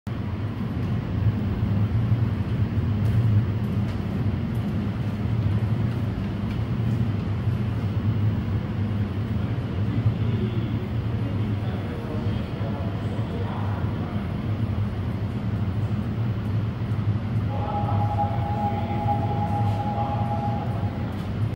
서울교통공사 4호선 당고개발 오이도행 이수역 대기
행선지 : 당고개발 오이도행

제작 : 현대로템

제작년도 : 2020년

편성 : 481편성

촬영장소 : 이수역

촬영날짜 : 2024년 6월 26일 오후 4시 58분